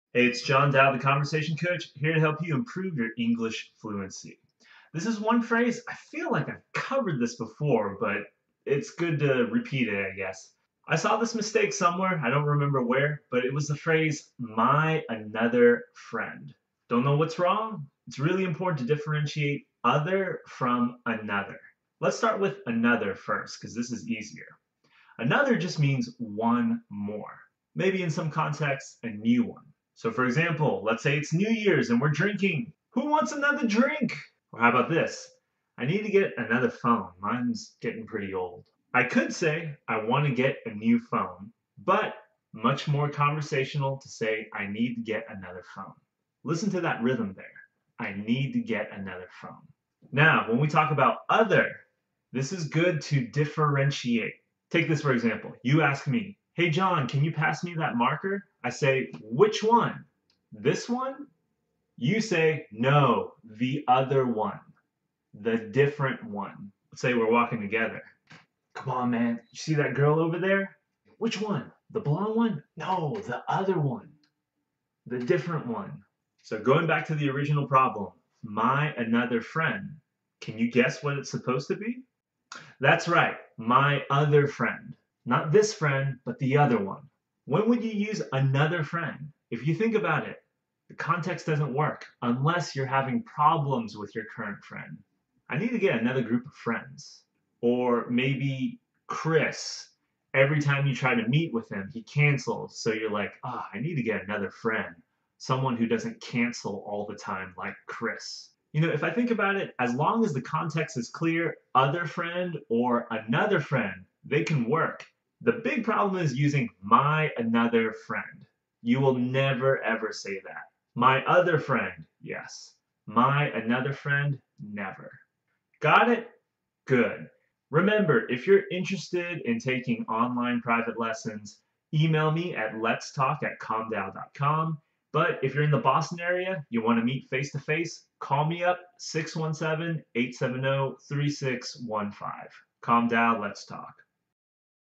What's the difference between "other" and "another"?
Do you confuse "other" with "another"? They do NOT mean the same thing. Learn how to use them properly in this week's English video.

Note: I didn't check my mic settings when recording, so the audio isn't the best. I subtitled everything I said to compensate.